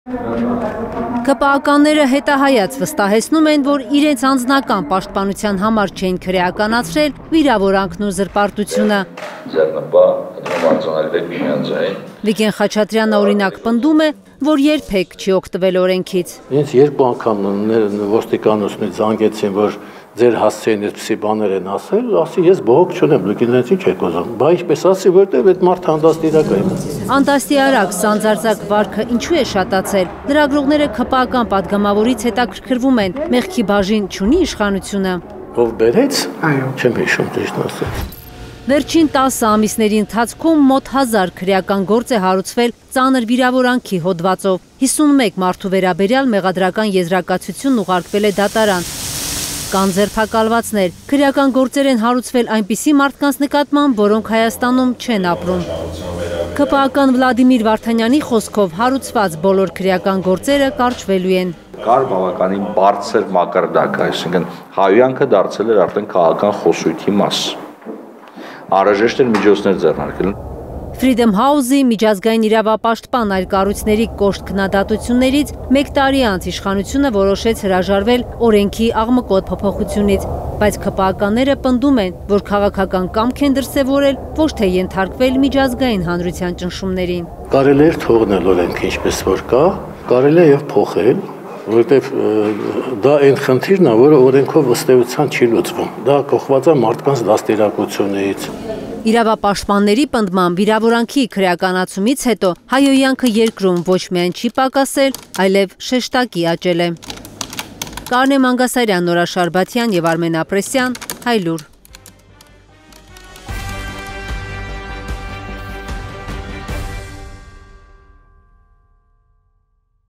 Հայհոյանքը երկրում ավելացել է. ծանր վիրավորանքի դեպքերով հարուցված մոտ 1000 քրգործերը կարճվելու են
Իրավապաշտպանները պնդում են, որ ծանր վիրավորանքի քրեականացումից հետո հայհոյանքը երկրում չի պակասել, հակառակը՝ աճել է: Անցած շաբաթ հայտնի դարձավ, որ ՔՊ-ն որոշել է ապաքրեականացնել այդ հոդվածը: